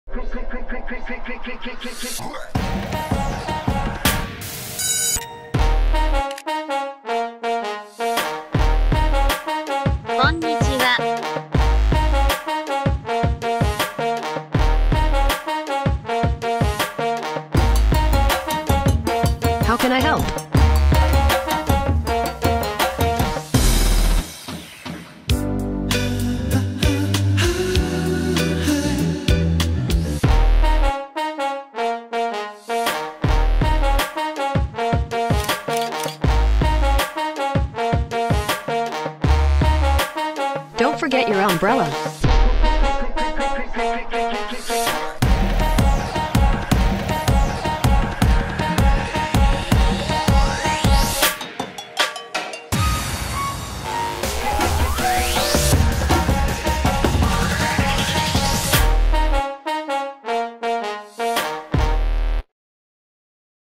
All Devices from Google Event October 2017
Here i show you all product from Google Event October 2017